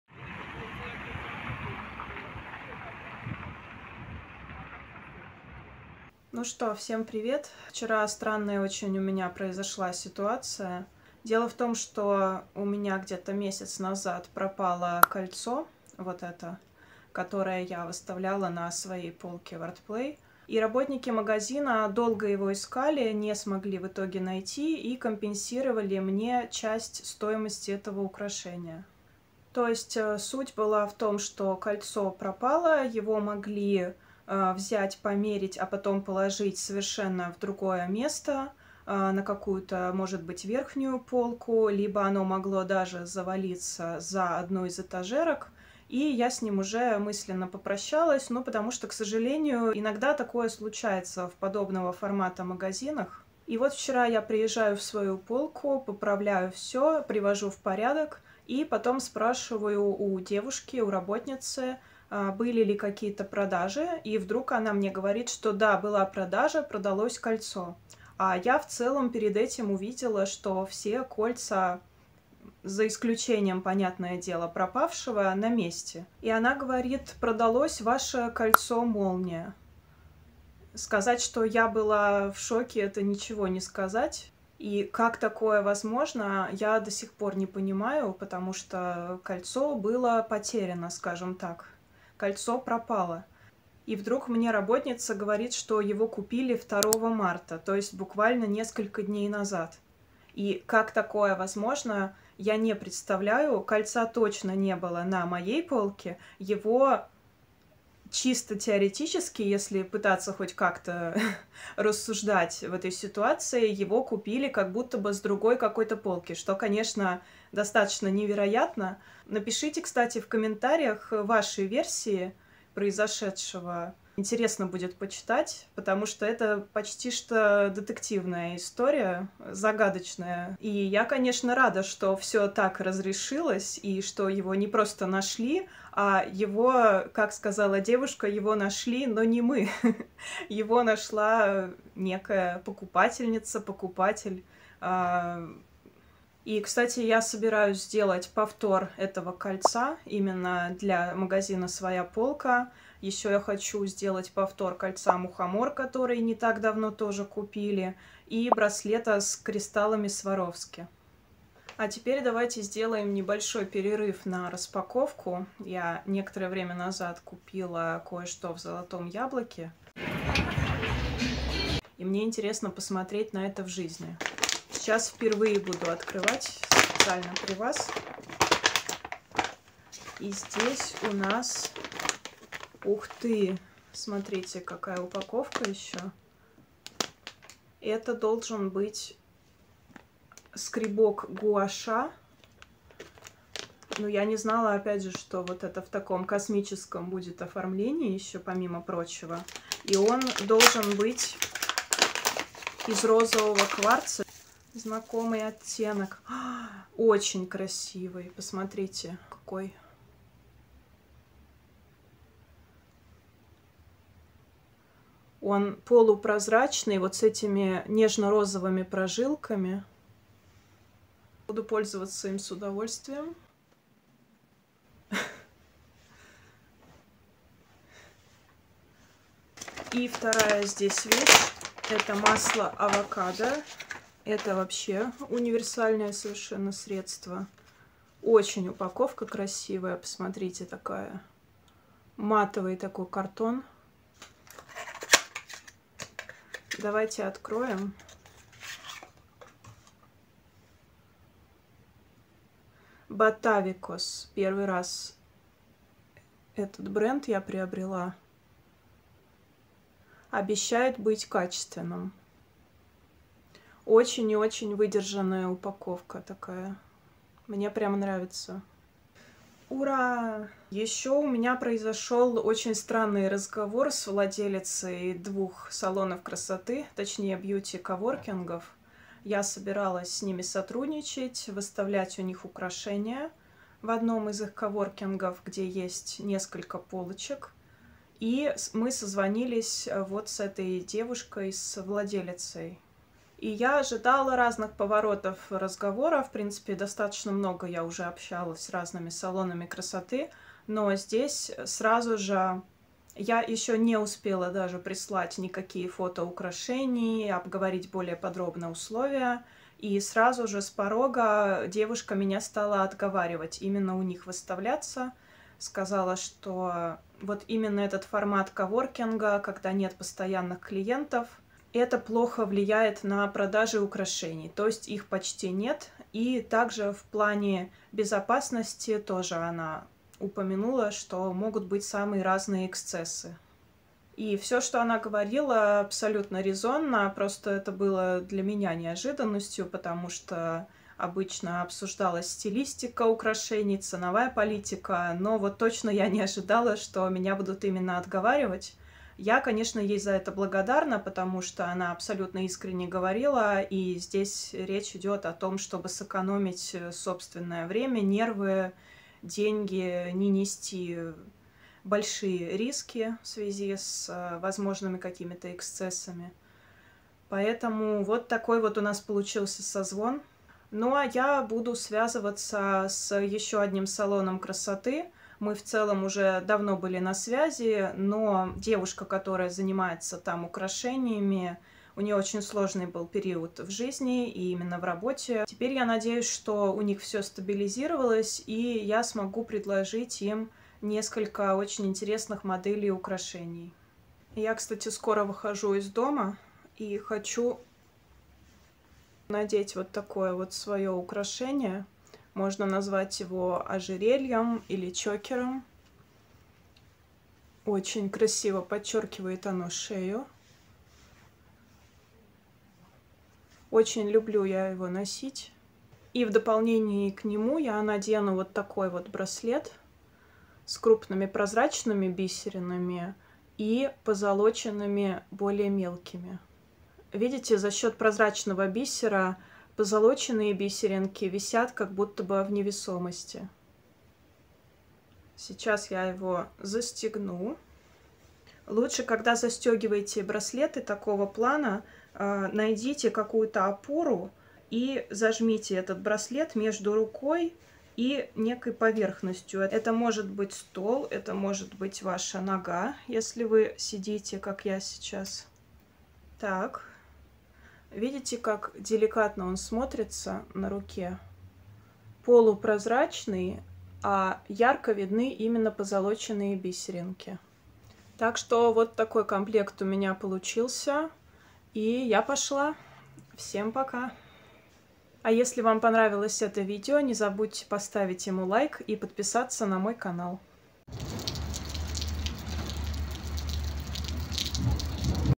У меня купили ПРОПАВШЕЕ КОЛЬЦО? Авторские украшения.
00:00 Солнечный день на Цветном
00:05 Исчезнувшее кольцо
2:26 Что я добавлю на полку в Артплей
2:41 Покупки из Золотого Яблока
4:30 Странный разговор с салоном красоты
6:35 Надеваю свои украшения ручной работы
7:19 Как легко застегнуть браслет?